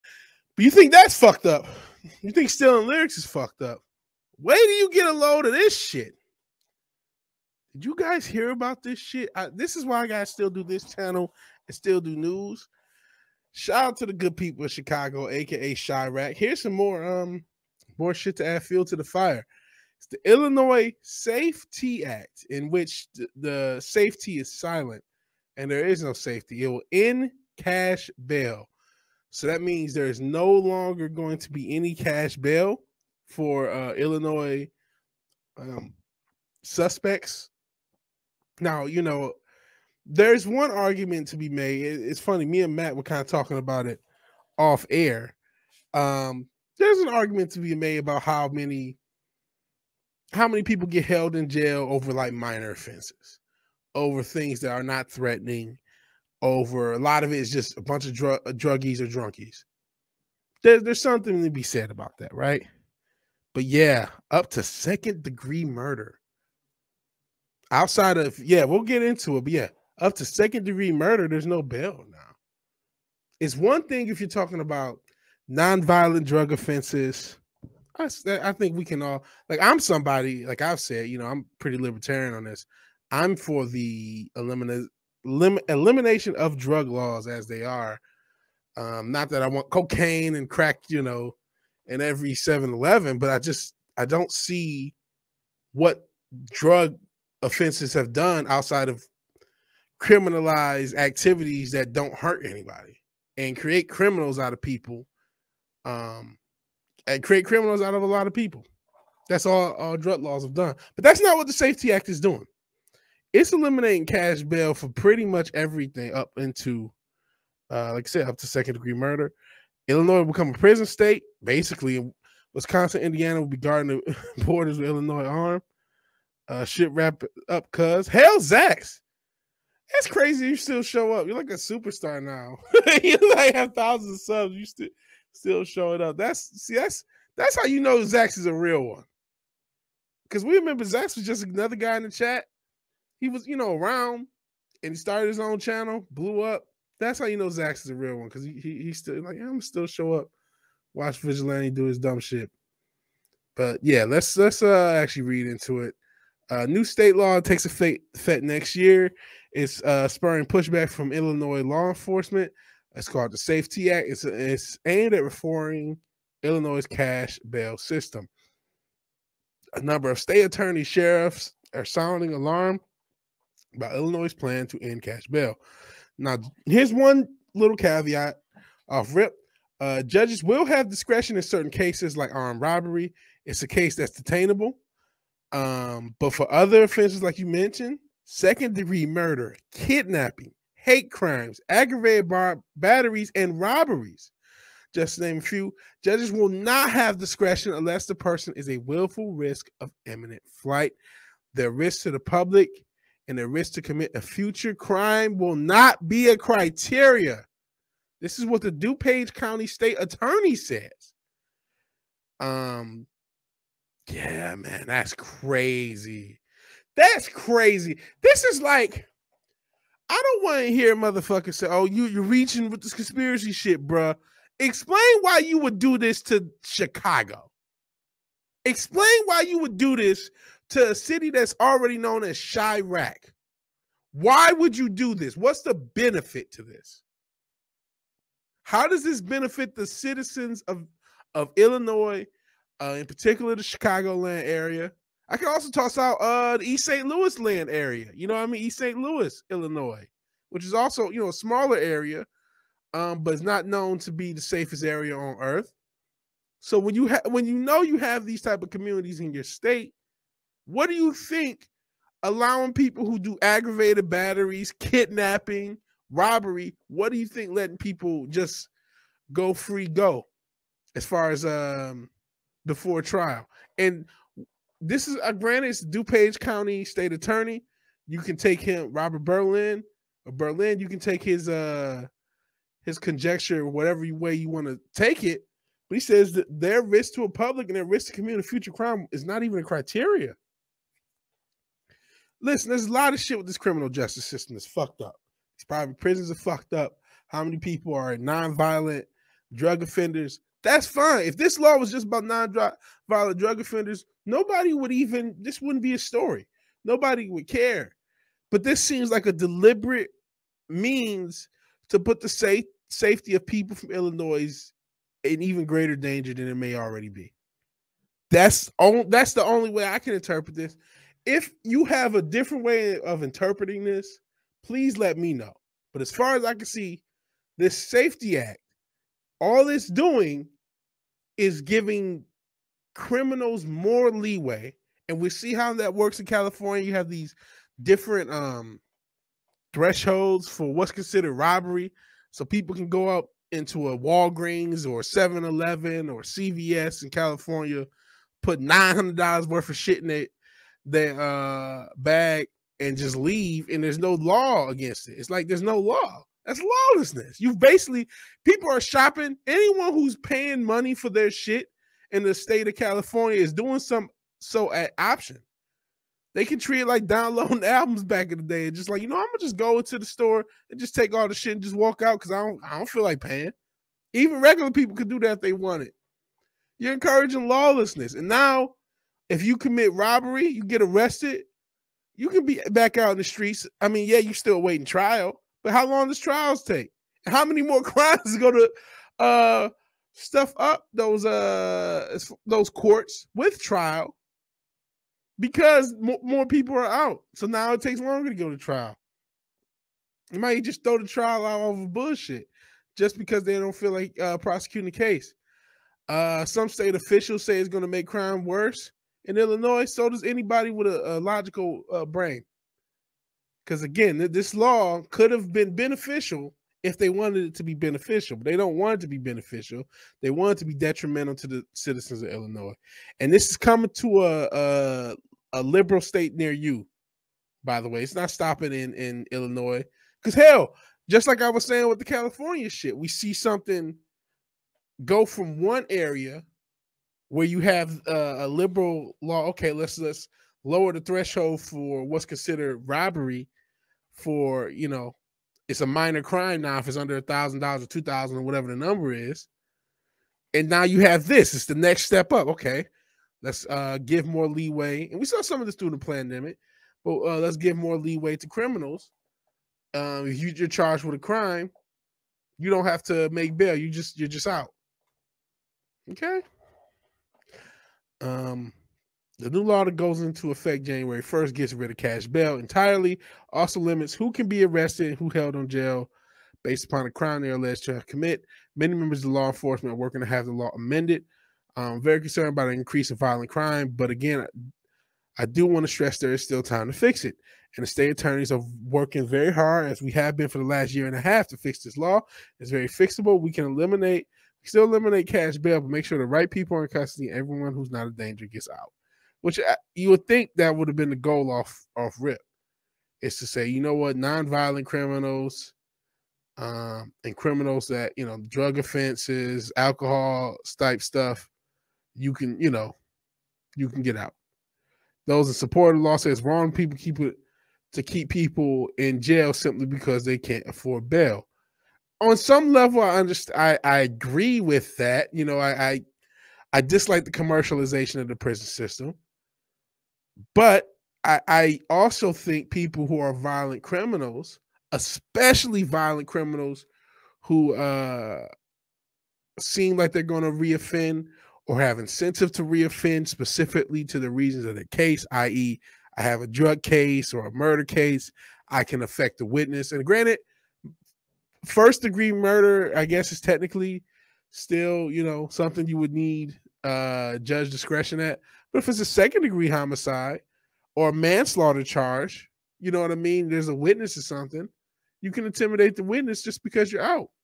No Safe-T in Illinois
This law makes no sense. The goal of eliminating bail can for nonviolent offenders can be done without an extreme law. This ironically also gives judges the ability to detain who they want.